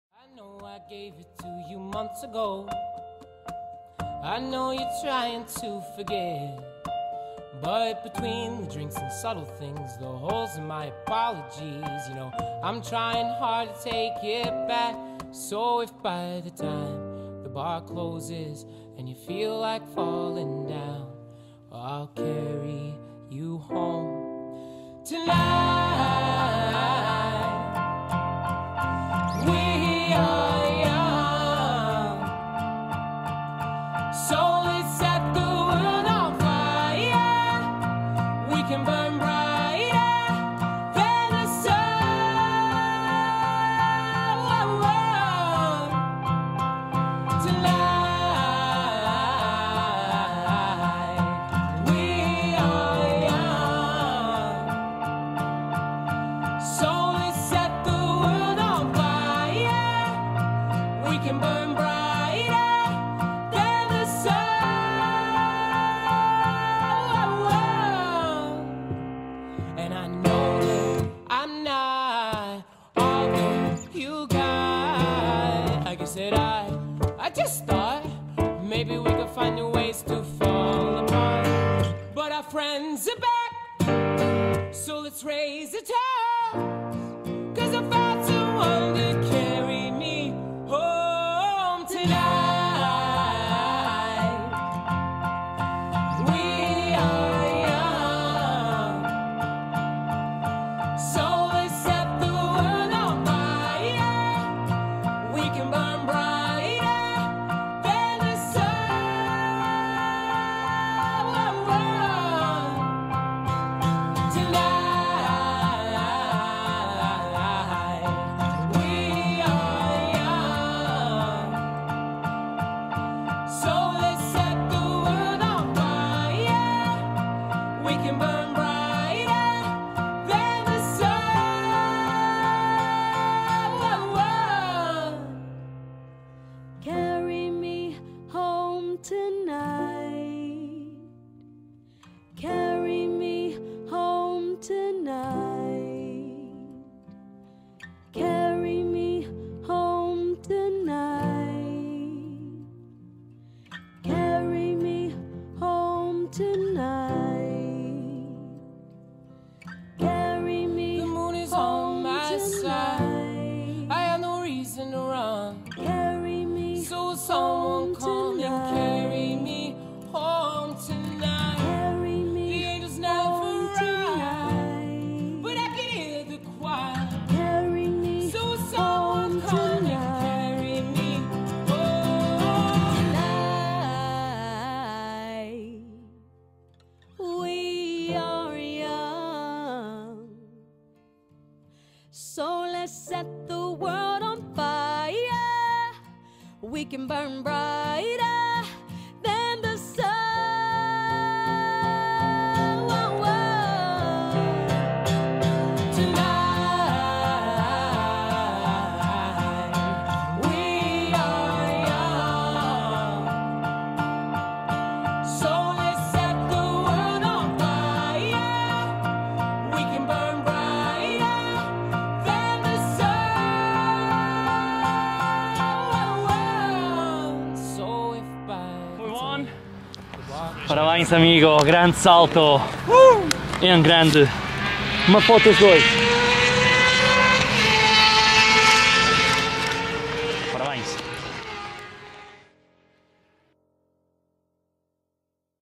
Voar sem Asas
Salto tandem nos céus de Braga. Setembro 2012.